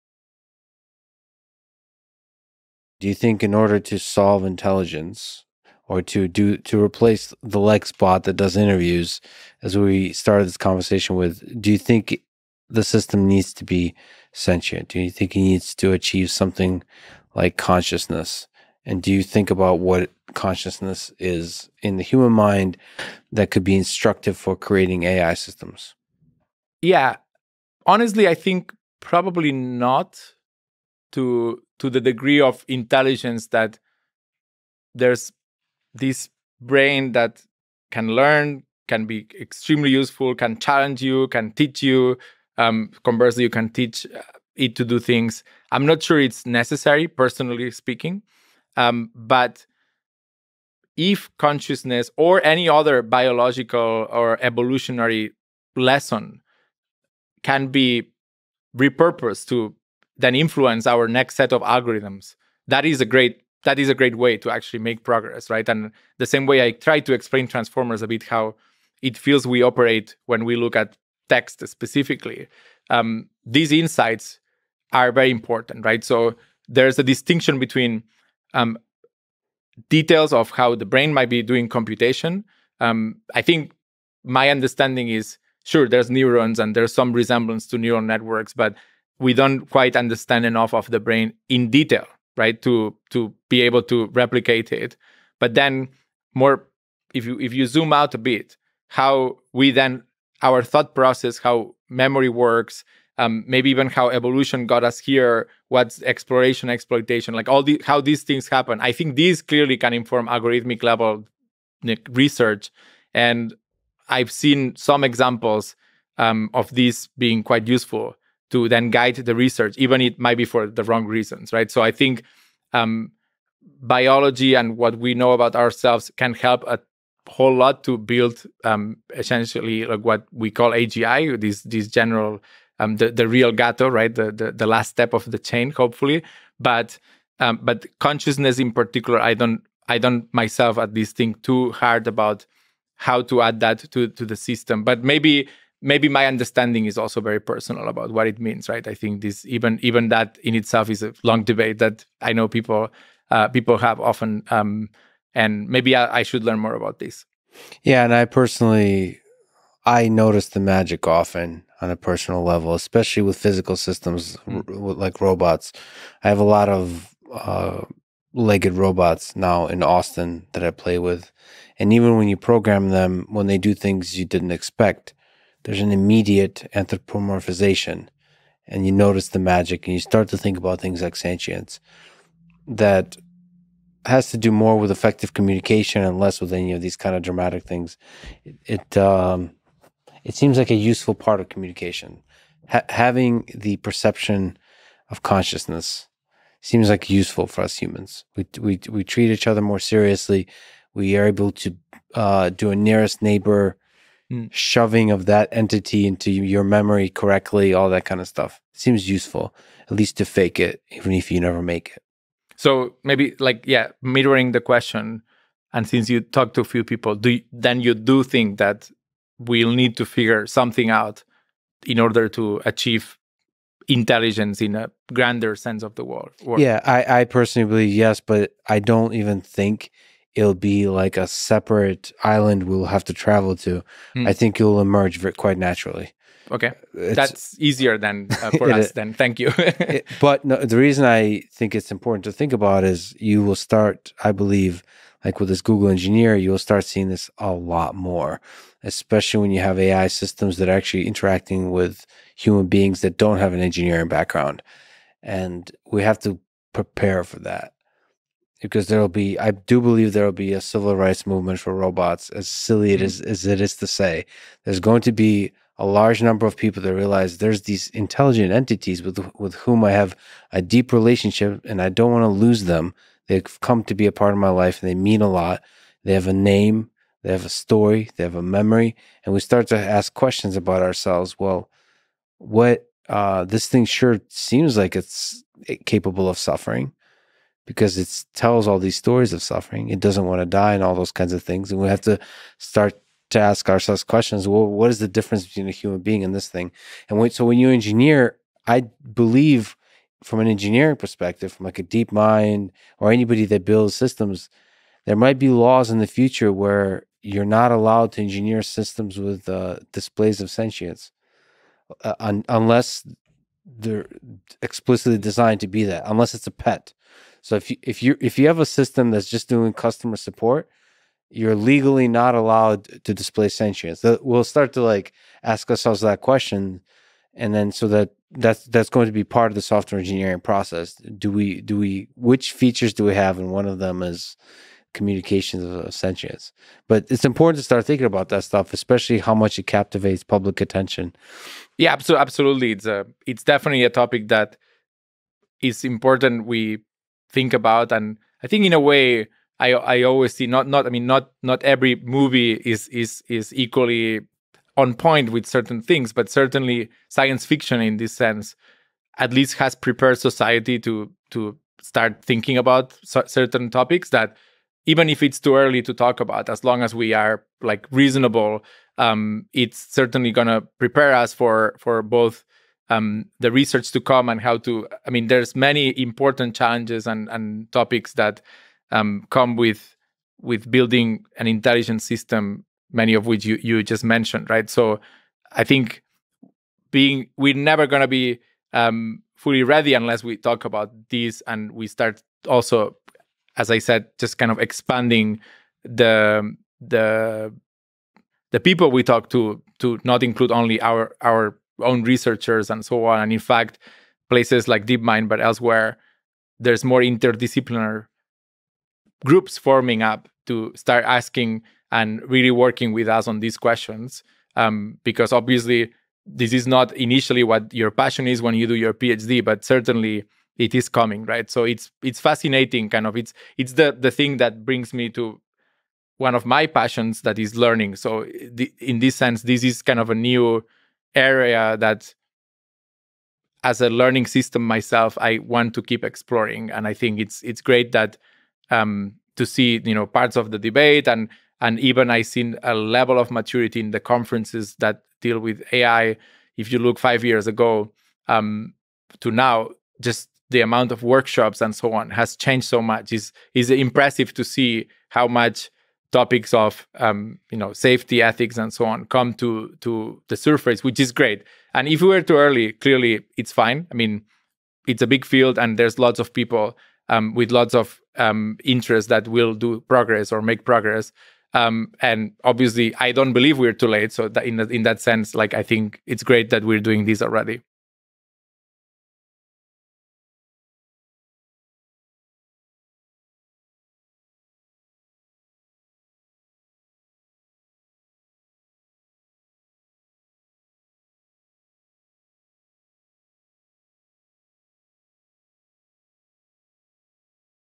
Does AI need to have consciousness? | Oriol Vinyals and Lex Fridman
GUEST BIO:
Oriol Vinyals is the Research Director and Deep Learning Lead at DeepMind.